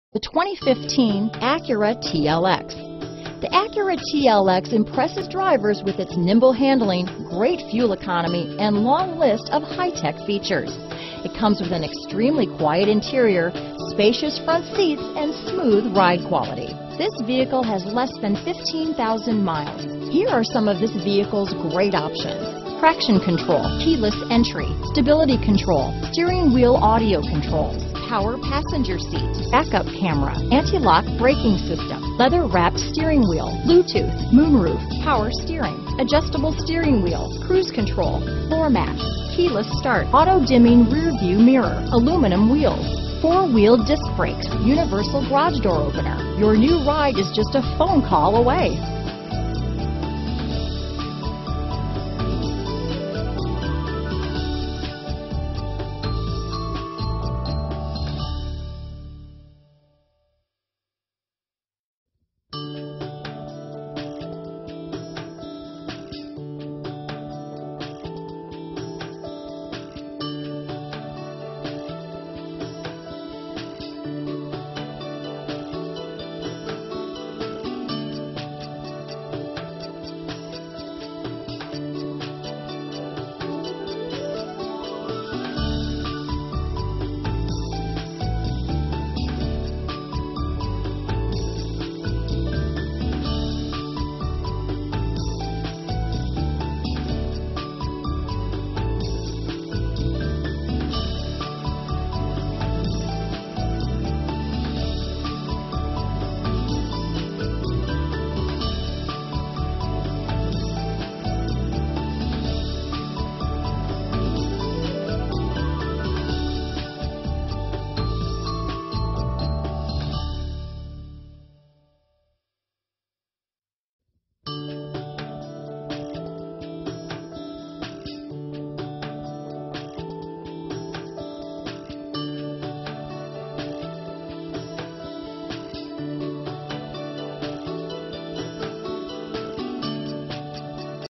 2015 Acura TLX Denver, Aurora, Centennial, Parker, Highlands Ranch, CO PA22064
2015 Acura TLX

Acura Certified and 3.5L V6 SOHC VTEC 24V. Switch to Mile High Honda! Perfect Color Combination! Do you want it all, especially low miles? Well, with this outstanding-looking 2015 Acura TLX, you are going to get it.. Acura Certified Pre-Owned means you not only get the reassurance of a 12mo/12,000 mile limited warranty, but also up to a 7yr/100k mile powertrain warranty, a 182-point inspection/reconditioning, 24/7 roadside assistance, trip-interruption services, rental car benefits, Acura Concierge Service, and a complete CARFAX vehicle history report. This car will take you where you need to go every time...all you have to do is steer! All prices include Dealer Handling fee of $499. Call us with any questions.